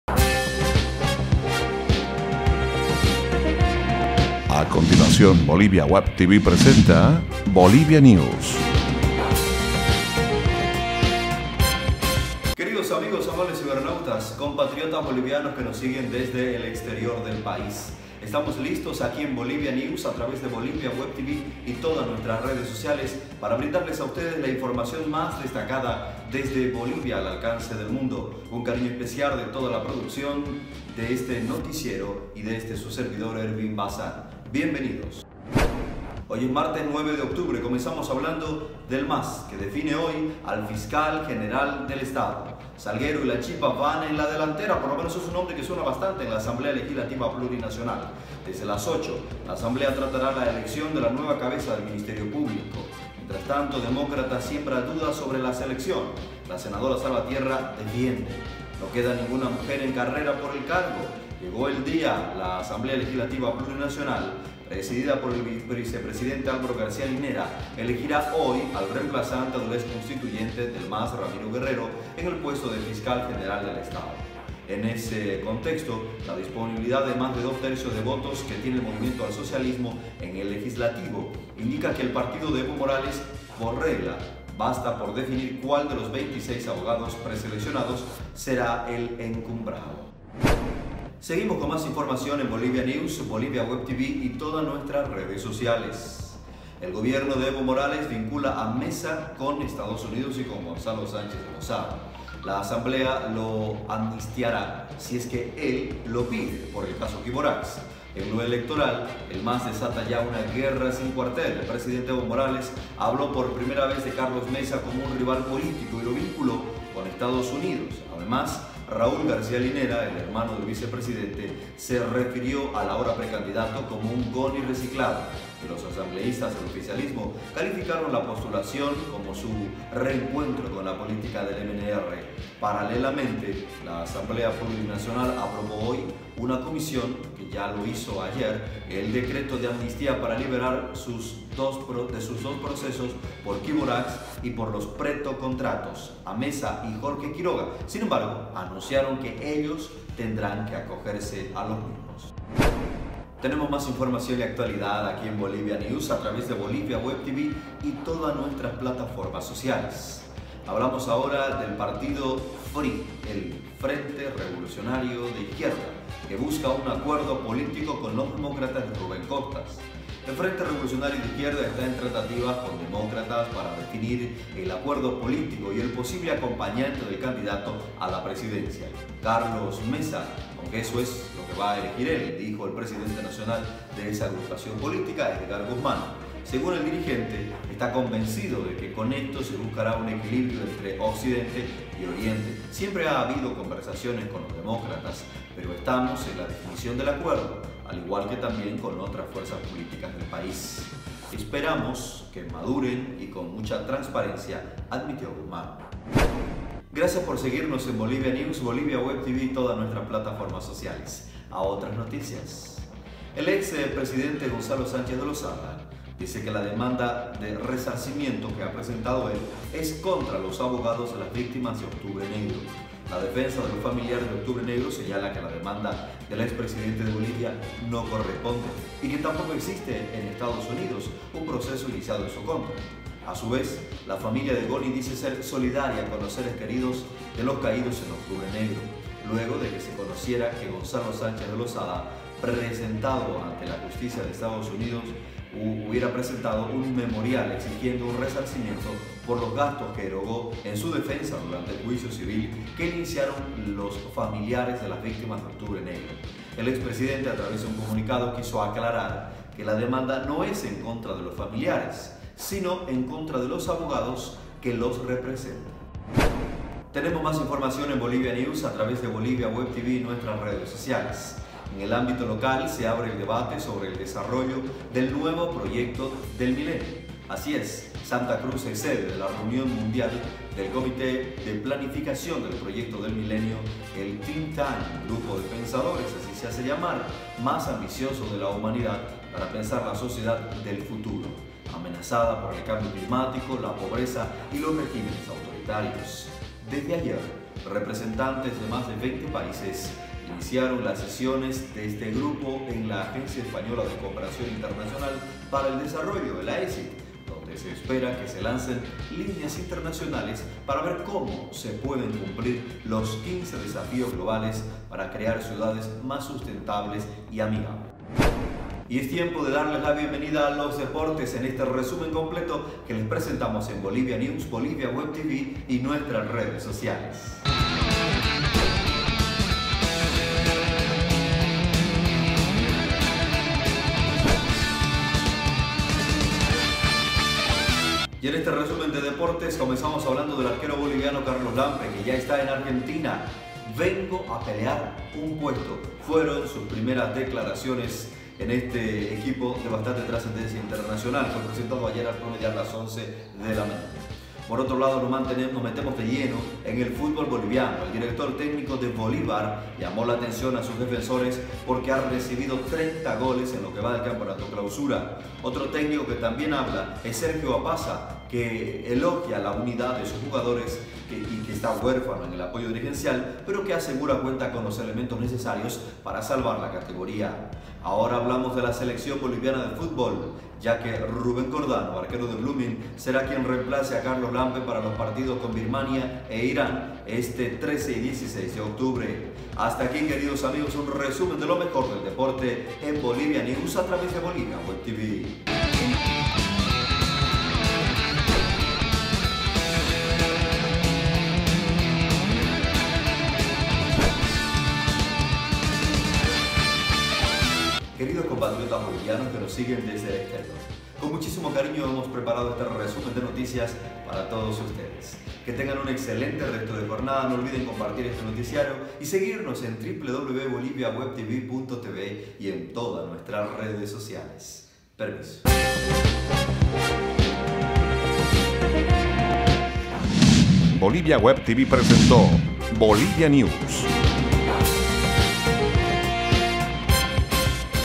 Últimas Noticias de Bolivia: Bolivia News, Martes 9 de Octubre 2018
-Comenzamos hablando que el MAS define hoy al fiscal general; Salguero y Lanchipa van en la delantera.
Desde las 8:00, la Asamblea tratará la elección de la nueva cabeza del Ministerio Público. Demócratas siembra dudas sobre la selección. La senadora Salvatierra la defiende. No queda ninguna mujer en carrera por el cargo.
Llegó el día. La Asamblea Legislativa Plurinacional, presidida por el vicepresidente Álvaro García Linera, elegirá hoy al reemplazante del exconstituyente del MAS Ramiro Guerrero en el puesto de fiscal general del Estado. En ese contexto, la disponibilidad de más de dos tercios de votos que tiene el Movimiento Al Socialismo (MAS) en el Legislativo indica que el partido de Evo Morales, por regla, basta para definir cuál de los 26 abogados preseleccionados será encumbrado.

- Seguimos  con Bolivia News. El Gobierno de Evo vincula a Mesa con EEUU y Goni.
La asamblea lo amnistiará, si es que él lo pide, por el caso Quiborax. En lo electoral, el MAS desata guerra sin cuartel.
El presidente Evo Morales habló por primera vez de Carlos de Mesa como un rival político y lo vinculó con Estados Unidos (EEUU). Además, Raúl García Linera, el hermano del vicepresidente, se refirió al ahora precandidato como un “Goni reciclado” y los asambleístas del oficialismo calificaron la postulación como su “reencuentro con la política del MNR”.
Paralelamente, la Asamblea Plurinacional aprobará hoy (una comisión ya lo hizo ayer) el decreto de amnistía para liberar de sus procesos por Quiborax y Petrocontratos a  Mesa y Jorge Quiroga, pero ellos tendrán que acogerse a él.

-Damos vuelco a la pagina  el partido del FRI busca acuerdo político con Demócratas de Rubén Costas.
El Frente Revolucionario de Izquierda (FRI) está en tratativas con Demócratas para definir el acuerdo político y el posible acompañante del candidato a la presidencia, Carlos de Mesa, aunque eso lo va elegir él, dijo el presidente nacional de esa agrupación política, Edgar Guzmán.
Según el dirigente, está convencido de que con esto se buscará un equilibrio entre Occidente y Oriente. “Siempre ha habido conversaciones con los Demócratas, pero estamos en la definición del acuerdo, al igual que también con otras fuerzas políticas del país. Esperamos que maduren y con mucha transparencia”, admitió Guzmán.

- Por otro lado Hablamos del Ex presidente Goni dice que la demanda de resarcimiento es contra los abogados de las víctimas de Octubre Negro.
La defensa de los familiares de Octubre Negro señala que la demanda del ex presidente de Bolivia no corresponde y que tampoco existe, en EEUU, un proceso iniciado en su contra. A su vez, la familia de Goni dice ser solidaria con los seres queridos los caídos en Octubre Negro
Luego de que se conociera que Gonzalo Sánchez de Lozada había presentado ante la justicia de EEUU un memorial exigiendo un resarcimiento por los gastos que erogó en su defensa durante el juicio civil que le iniciaron las familias de las víctimas de Octubre Negro, el ex presidente -a través de un comunicado- quiso aclarar que la demanda no es en contra de los familiares, sino en contra de los abogados que los representan.

- Y en el ámbito local  se abre el debate sobre el desarrollo del nuevo proyecto del milenio.
Santa Cruz es la sede de la reunión mundial del Comité de Planificación del Proyecto del Milenio (Millennium Project), el think-tank (grupo de pensadores) más ambicioso de la Humanidad para pensar la sociedad del futuro, amenazada por el cambio climático, la pobreza y los regímenes autoritarios.
Desde ayer, representantes de más de 20 países iniciaron las sesiones del MP en Ia Agencia Española de Cooperación Internacional para el Desarrollo (AECID) donde se espera que se lancen líneas internacionales para ver cómo se pueden cumplir los 15 Desafíos Globales para crear ciudades más sustentables y amigables.

- Y ES TIEMPO  DE  DEPORTES EN BOLIVIANEWS.
El arquero boliviano Carlos Lampe ya esta en argentina vengo a pelear un puesto fueron sus primeras declaraciones.
Por otro lado nos metemos de lleno en el futbol boliviano. El DT de Bolívar llama la atención a sus defensores porque a recibido 30 goles en lo que va del campeonato clausura. Otro técnico que también habla es Sergio Apaza que elogia la unidad de sus jugadores que esta huérfano en el apoyo dirigencial pero que cuenta con los elementos para salvar la categoría.
Ahora hablamos de la Selección ya que Rubén Cordano arquero de Blooming reemplazara a Carlos Lampe para los partidos con Birmania y Irán este 13 y 16 de Octubre.